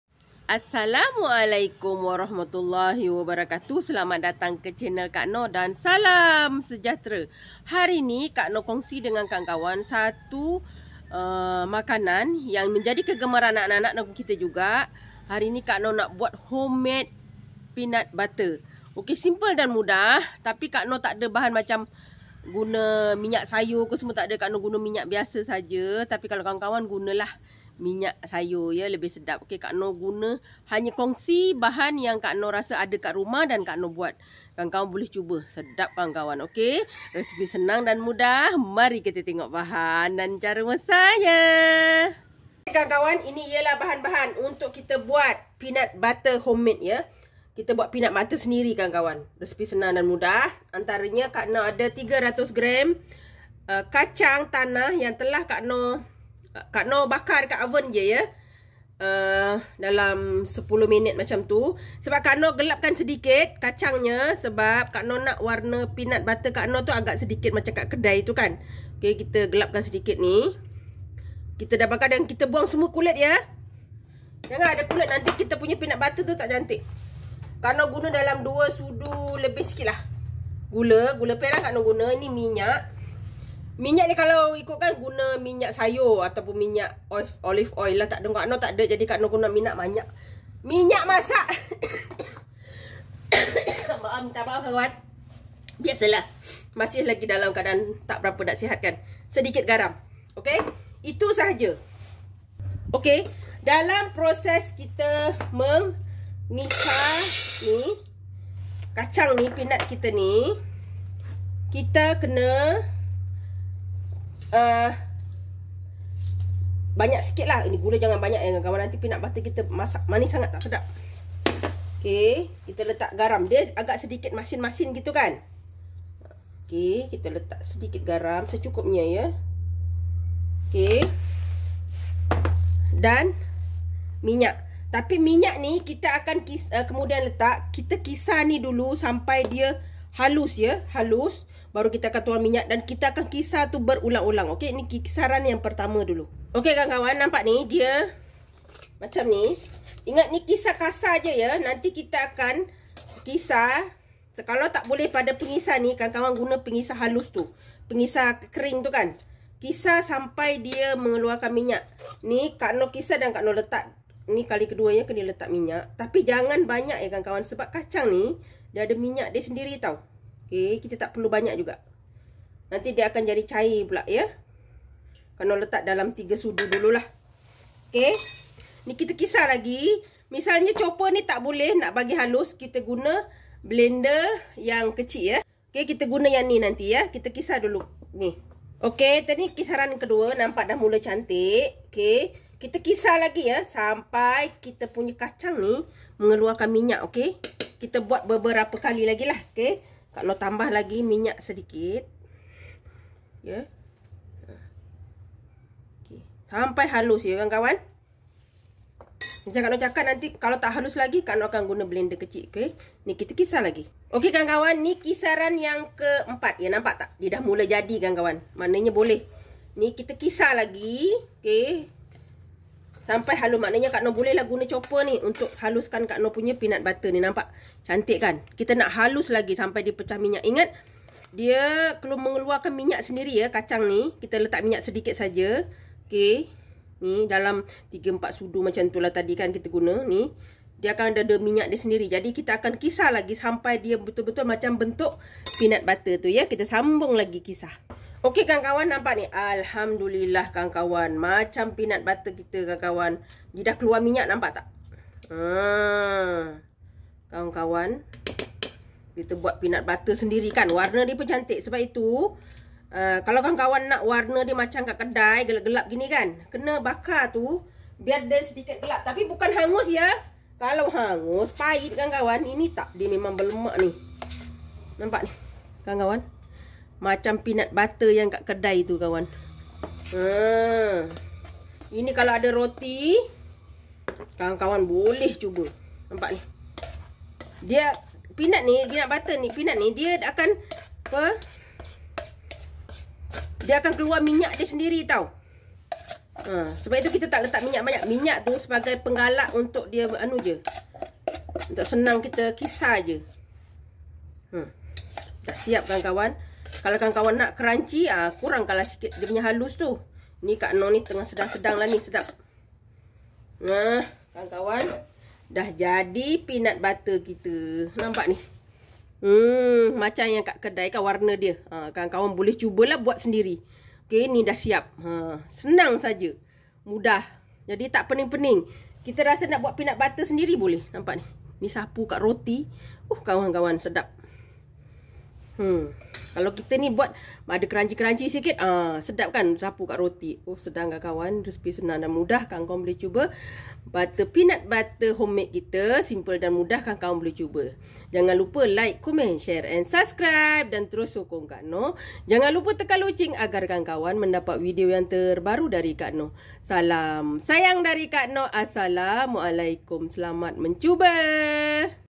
cara buat peanut butter homemade by kak nur @tip warna cantik
bahan2 nya
300 gram kacang tanah( di bakar 10 minit sedikit gelap
3 sb gula
5 sb minyak( seelok2 nya minyak sayur
sedikit garam

kisar beerahap2 selama 5x hingga betul2  keluar minyak ..kisar jgn terus2 takut mesin rosak